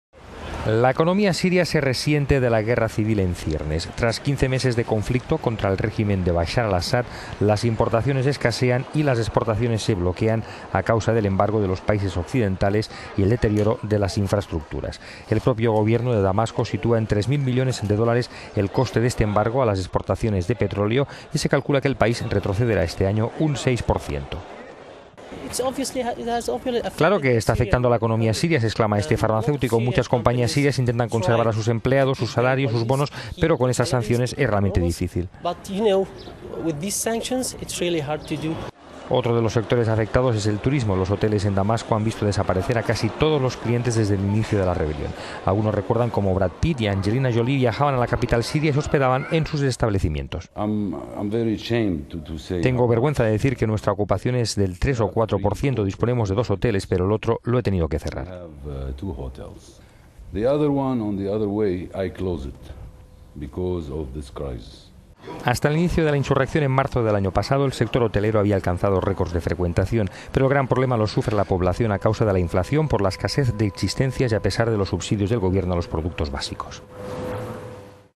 La economía siria retrocede por la guerra civil
La economía siria se resiente de la guerra civil en ciernes. Tras quince meses de conflicto contra el régimen de Bachar al Asad, las importaciones escasean y las exportaciones se bloquean a causa del embargo de los países occidentales y el deterioro de las infraestructuras. El propio gobierno de Damasco sitúa en 3.000 millones de euros el coste de este embargo a las exportaciones de petróleo y se calcula que el país retrocederá este año un 6%

"Claro que está afectando a la economía siria", se exclama Mazen, farmacéutico. "Muchas compañías sirias intentar conservar a sus empleados, sus salarios, sus bonos, pero con estas sanciones es realmente difícil".

Otro de los sectores afectados es el turismo. Los hoteles en Damasco han visto desaparecer a casi todos los clientes desde el inicio de la rebelión. Algunos recuerdan como Brad Pitt y Angelina Jolie viajaban a la capital siria y se hospedaban en sus establecimientos.

"Tengo vergüenza de decir que nuestra ocupación es del tres o cuatro por ciento", reconoce Fayez al-Midani. "Disponemos de dos hoteles, pero el otro lo he tenido que cerrar".

Hasta el inicio de la insurrección, en marzo del año pasado, el sector hotelero había alcanzado récords de frecuentación. Pero el gran problema lo sufre la población a causa de la inflación por la escasez de existencias a pesar de los subsidios del gobierno a los productos básicos.